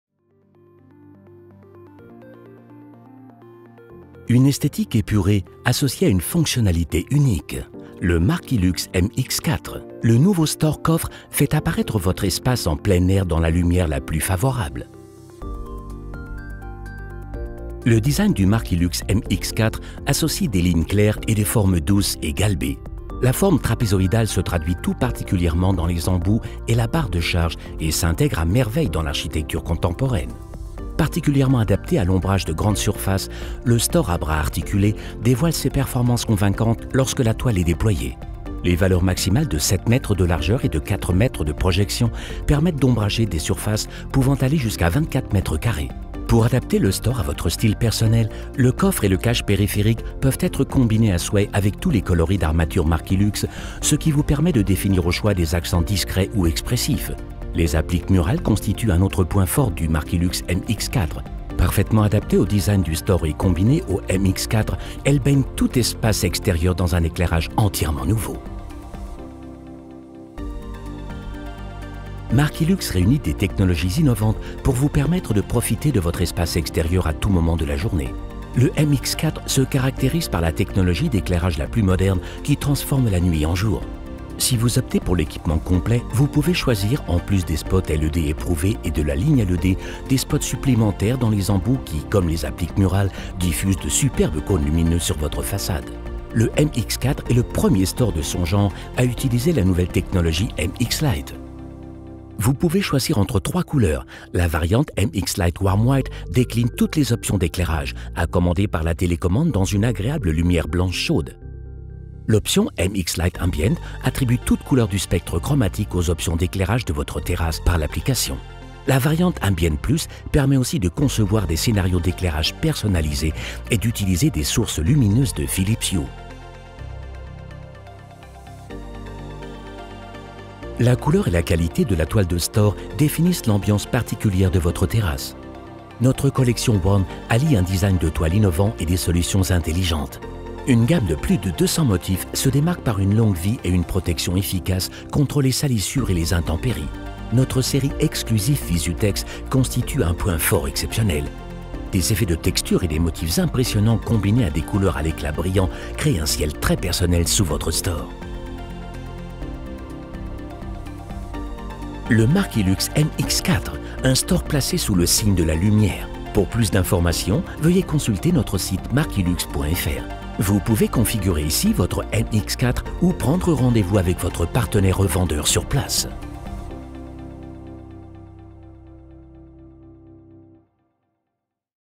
markilux MX-4 - Store banne coffre
Place au nouveau store coffre markilux MX-4. Conçu pour l'ombrage de grandes surfaces, il convainc par son design avant-gardiste et le cache périphérique. Le design du produit - esthétique et fonctionnel. Avec ses options d'éclairage innovantes et ses appliques supplémentaires, il brille aux heures du soir, créant une atmosphère d'éclairage spectaculaire et magique. Lever de rideau pour une belle composition.

Timecodes
0:00 INTRO
0:15 DESIGN
1:10 TECHNIQUE
2:08 TOILE DE STORE
2:40 OUTRO